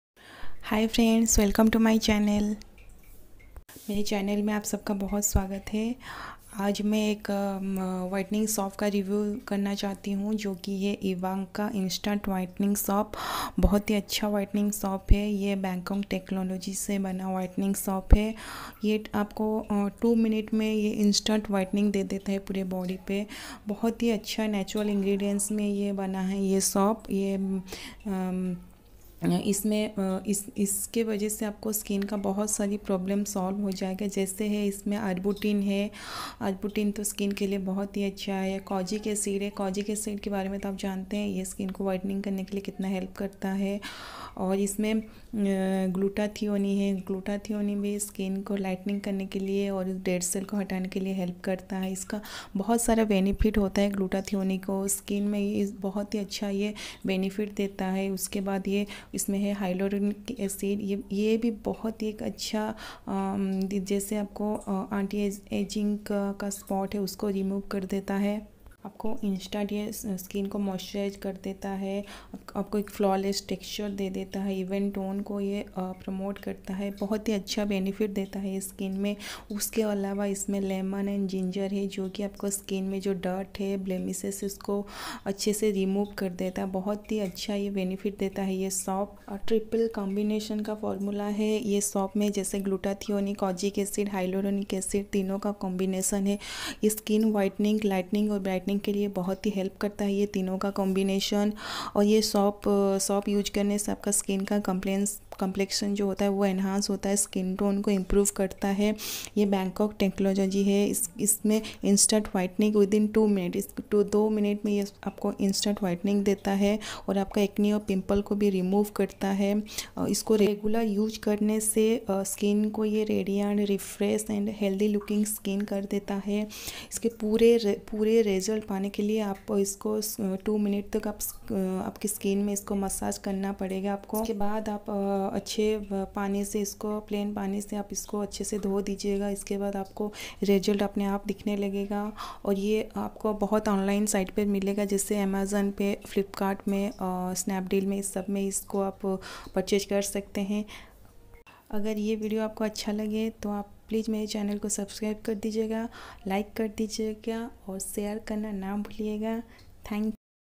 Ivanka instant whitening soap|| 2 minute में पूरा body को गोरा बना दे!!
IVANKA soap gives skin whitening lightening and brightening effect with its triple combination formula G-lutathione, K-ojic Acid and H-yaluronic Acid Lemon & Ginger It removes dirt, impurities and reduces blemishes without irritating the skin. IVANKA soap helps to improve the skin complexion, elasticity & skin tone.